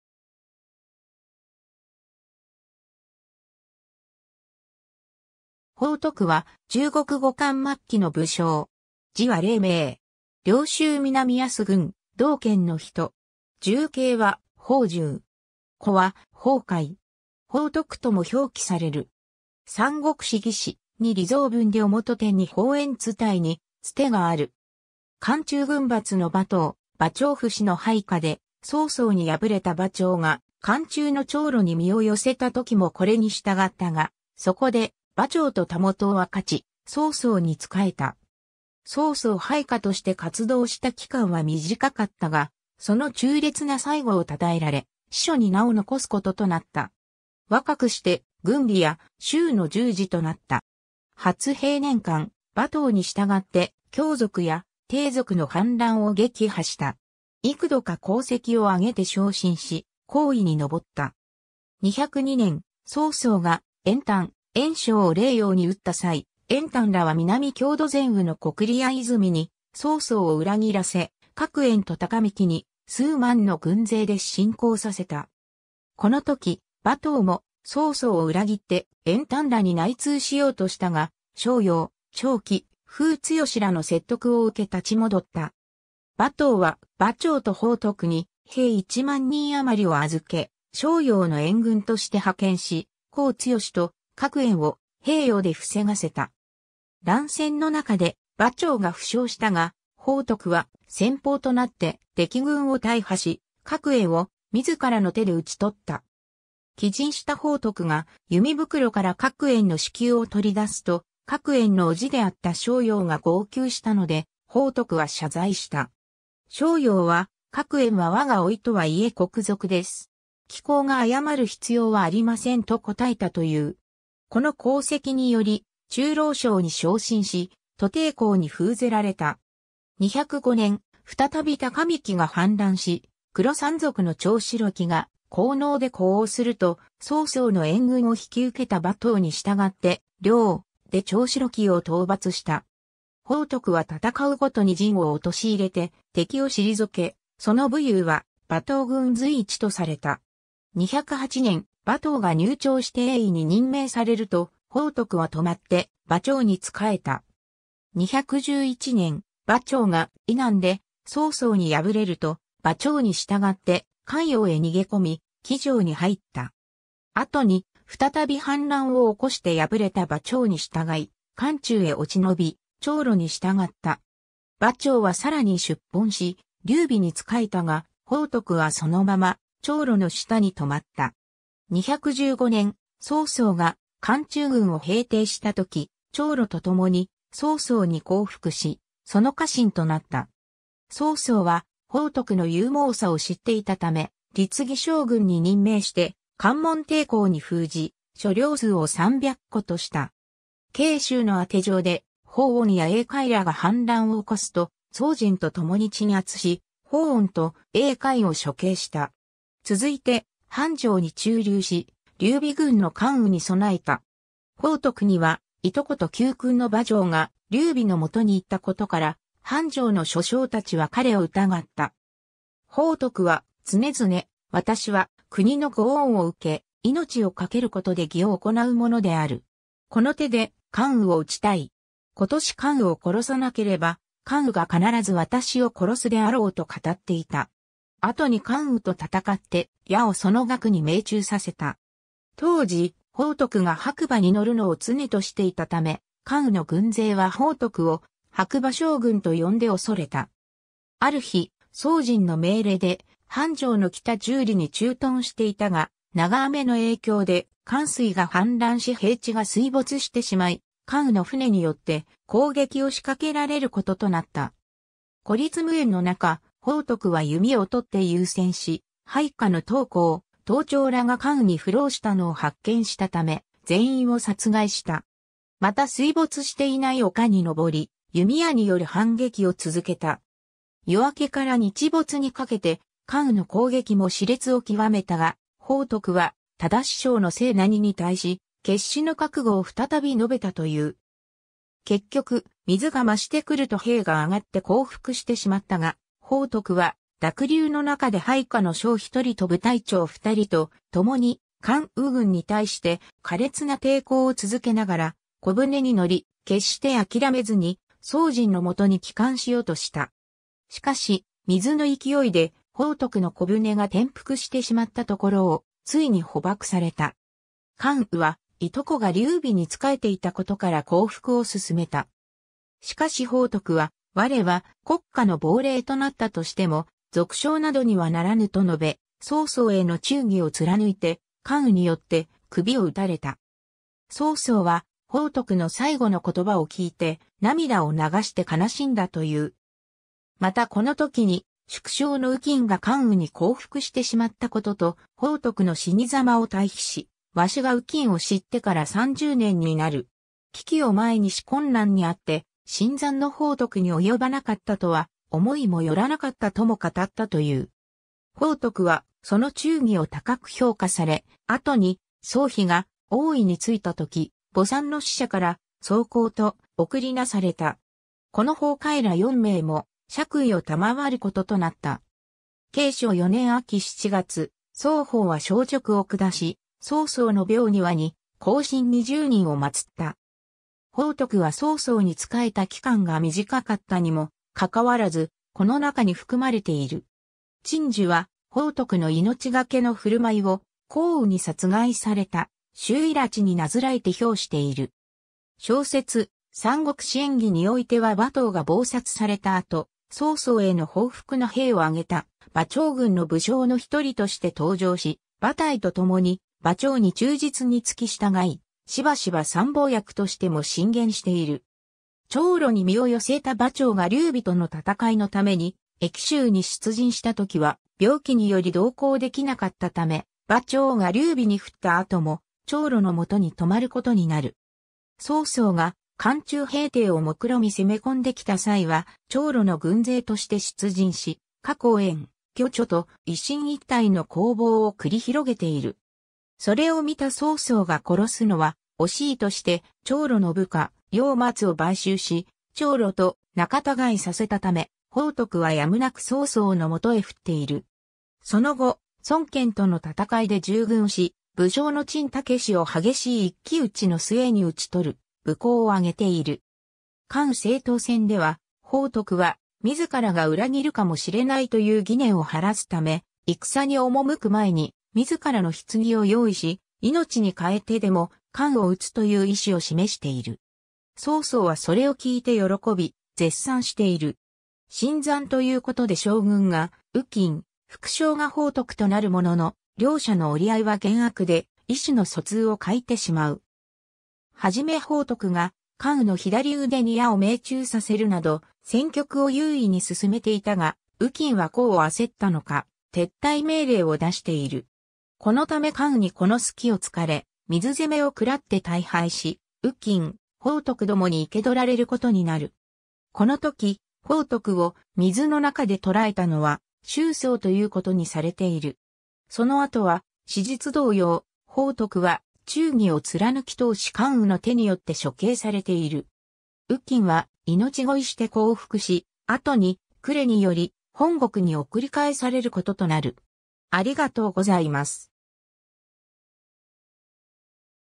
龐徳
龐 徳（ほう とく、170年? - 219年8月）は、中国後漢末期の武将。
字は令明（れいめい）。
涼州南安郡豲道県（現在の甘粛省天水市武山県四門鎮）の人。
従兄は龐柔。
子は龐会。
龐悳とも表記される。
『三国志』魏志「二李臧文呂許典二龐閻伝」に伝がある。
 関中軍閥の馬騰・馬超父子の配下で、曹操に敗れた馬超が漢中の張魯に身を寄せた時もこれに従ったが、そこで馬超と袂を分かち曹操に仕えた。
曹操配下として活動した期間は短かったが、その忠烈な最期を称えられ、史書に名を残す事となった。
 若くして郡吏や州の従事となった。
 初平年間、馬騰に従って羌族や氐族の反乱を撃破した。
幾度か功績を挙げて昇進し、校尉に昇った。
 202年、曹操が袁譚・袁尚を黎陽に討った際、袁譚らは南匈奴単于の呼廚泉に曹操を裏切らせ、郭援と高幹に数万の軍勢で侵攻させた。
この時、馬騰も曹操を裏切って袁譚らに内通しようとしたが、鍾繇・張既・傅幹らの説得を受け立ち戻った。
 馬騰は馬超と龐徳に兵一万人余りを預け、鍾繇の援軍として派遣し、高幹と郭援を平陽で防がせた。
乱戦の中で馬超が負傷したが、龐徳は先鋒となって敵軍を大破し、郭援を自らの手で討ち取った。
帰陣した龐徳が弓袋から郭援の首級を取り出すと、郭援の叔父であった鍾繇が号泣したので、龐徳は謝罪した。
鍾繇は「郭援は我が甥とはいえ国賊です。貴公が謝る必要はありません」と答えたという（『魏略』）。
この功績により中郎将に昇進し、都亭侯に封ぜられた。
 205年、再び高幹が反乱し、黒山賊の張白騎（張晟）が弘農で呼応すると、曹操の援軍を引き受けた馬騰に従って、両崤で張白騎を討伐した。
龐徳は戦う毎に陣を陥れて敵を退け、その武勇は馬騰軍随一とされた。
 208年、馬騰が入朝して衛尉に任命されると、龐徳は留まって馬超に仕えた。
 211年、馬超が渭南で曹操に敗れると、馬超に従って漢陽へ逃げ込み、冀城に入った。
後に、再び反乱を起こして敗れた馬超に従い、漢中へ落ち延び張魯に従った。
馬超は更に出奔し劉備に仕えたが、龐徳はそのまま張魯の下に留まった。
 215年、曹操が漢中郡を平定した時、張魯と共に曹操に降伏し、その家臣となった。
曹操は龐徳の勇猛さを知っていたため、立義将軍に任命して関門亭侯に封じ、所領数を300戸とした。
 荊州の宛城で侯音や衛開らが反乱を起こすと、曹仁と共に鎮圧し侯音と衛開を処刑した。
続いて樊城に駐留し、劉備軍の関羽に備えた。
 龐徳には、従兄と旧君の馬超が劉備の元にいたことから、樊城の諸将達は彼を疑った。
龐徳は常々「私は国のご恩を受け、命を懸ける事で義を行なうものである。この手で関羽を討ちたい。今年関羽を殺さなければ、関羽が必ず私を殺すであろう」と語っていた。
後に関羽と戦って矢をその額に命中させた。
当時、龐徳が白馬に乗るのを常としていたため、関羽の軍勢は龐徳を白馬将軍と呼んで畏れた。
 ある日、曹仁の命令で樊城の北10里に駐屯していたが、長雨の影響で漢水が氾濫し平地が水没してしまい、関羽の船によって攻撃を仕掛けられる事となった。
孤立無援の中、龐徳は弓をとって勇戦し、配下の董衡・董超らが関羽に降ろうしたのを発見したため、全員を殺害した。
また水没していない丘に登り、弓矢による反撃を続けた。
夜明けから日没にかけて、関羽の攻撃も熾烈を極めたが、龐徳は督将の成何に対し、決死の覚悟を再び述べたという。
結局、水が増してくると兵が挙って降伏してしまったが、龐徳は濁流の中で配下の将一人と部隊長二人と共に、関羽軍に対して苛烈な抵抗を続けながら小舟に乗り、決して諦めずに曹仁の元に帰還しようとした。
しかし、水の勢いで龐徳の小舟が転覆してしまったところを、ついに捕縛された。
 関羽は従兄が劉備に仕えていた事から降伏を勧めた。
しかし龐徳は「我は国家の亡霊となったとしても、賊将などにはならぬ」と述べ、曹操への忠義を貫いて関羽によって首を討たれた。
 曹操は、龐徳の最期の言葉を聞いて涙を流して悲しんだという。
またこの時に、宿将の于禁が関羽に降伏してしまった事と、龐徳の死に様を対比し「わしが于禁を知ってから30年になる。危機を前にし困難に遭って、（忠義を貫いた）新参の龐徳に及ばなかったとは思いもよらなかった」とも語ったという（「于禁伝」）。
 龐徳はその忠義を高く評価され、後に曹丕が王位に就いた時、墓参の使者から壮侯と諡された。
子の龐会ら4名も爵位を賜る事となった。
 景初4年（243年）秋7月、...